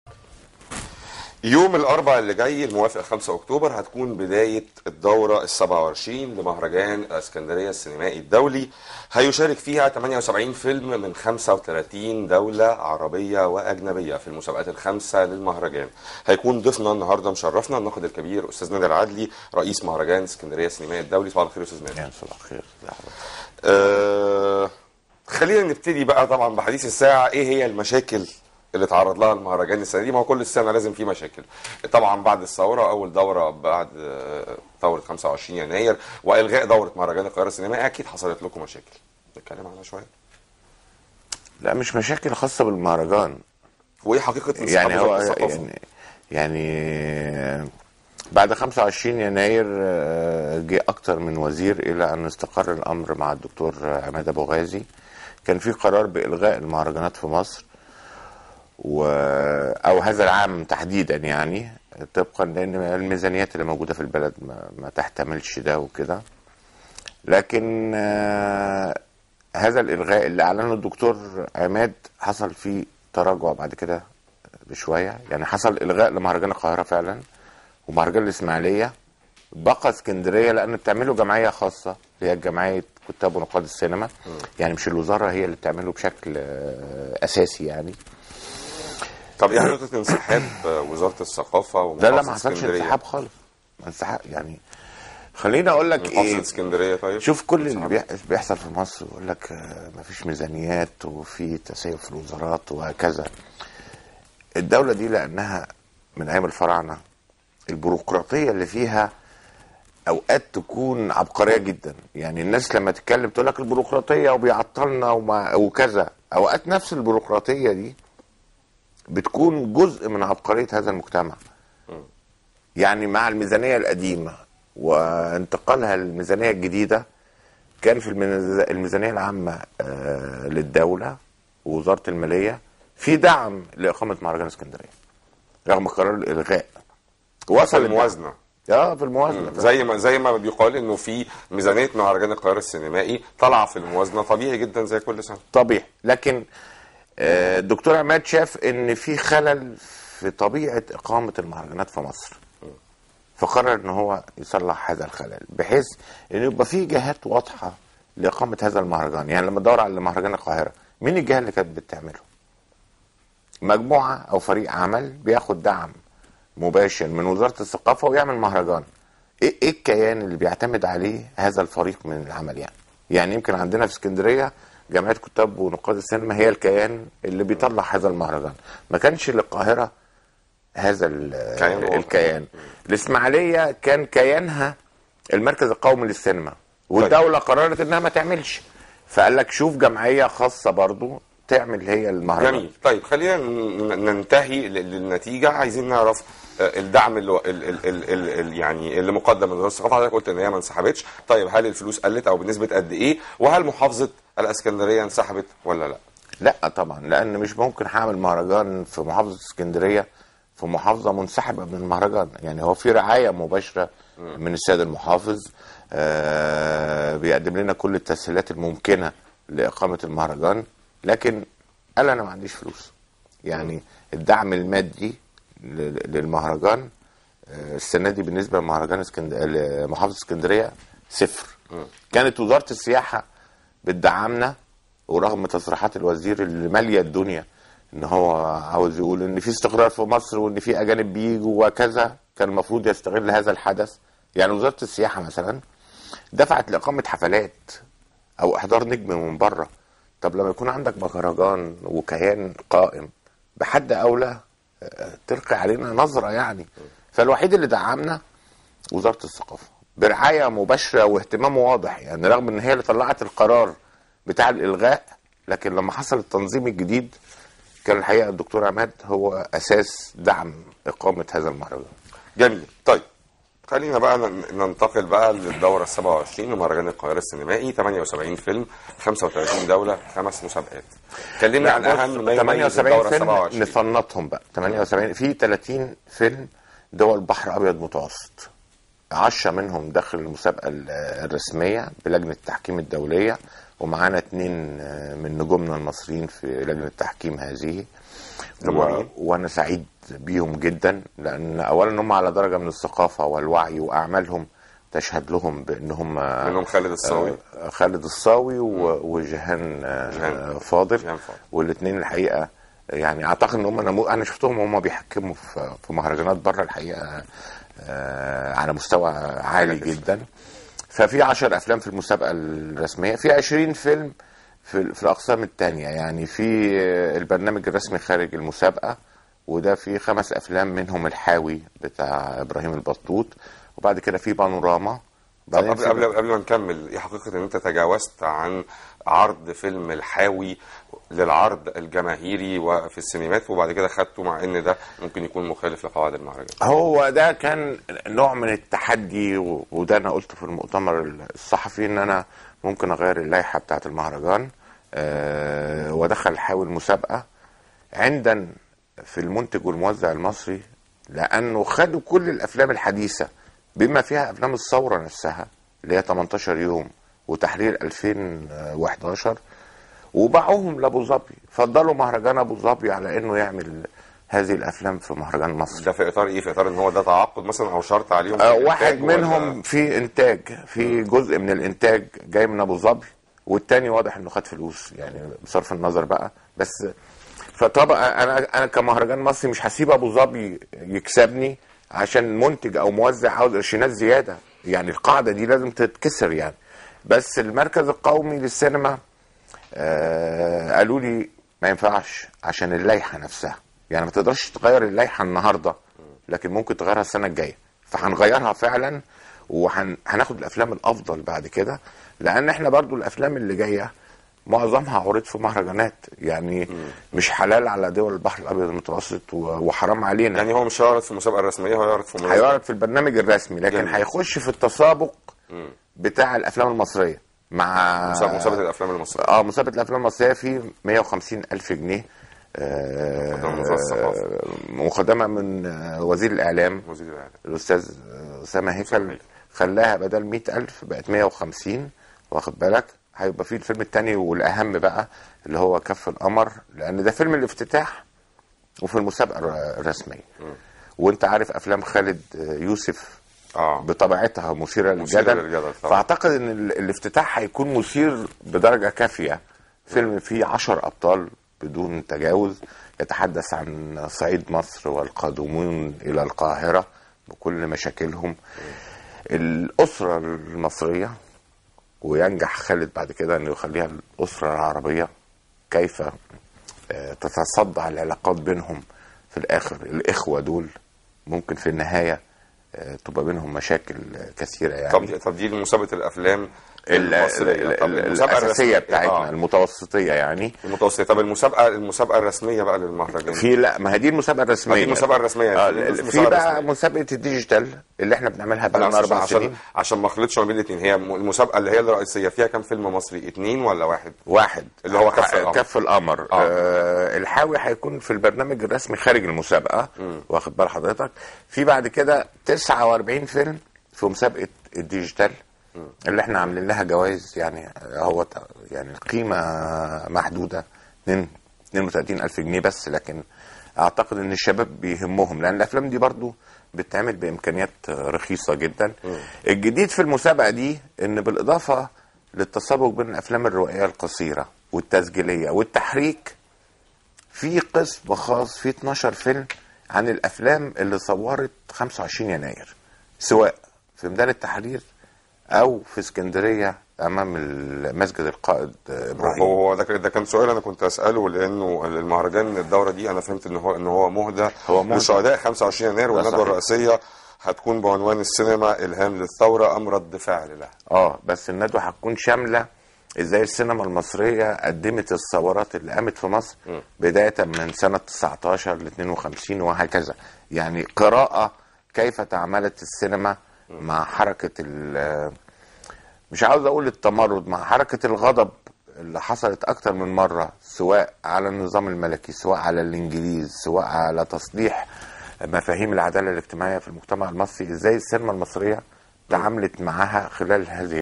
الدورة ال 27 لمهرجان الاسكندرية السينيمائى الدولى (1)
حوار مع الناقد السينيمائى أستاذ نادر عادلى رئيس مهرجان الالسكندرية السينيمائى الدولى حول الدورة ال 27 لمهرجان الاسكندرية السينيمائى الدولى التى سوف تقام الأربعاء القادم الموافق خ أكتوبر 2011
حلقة الأحد 2 أكتوبر 2011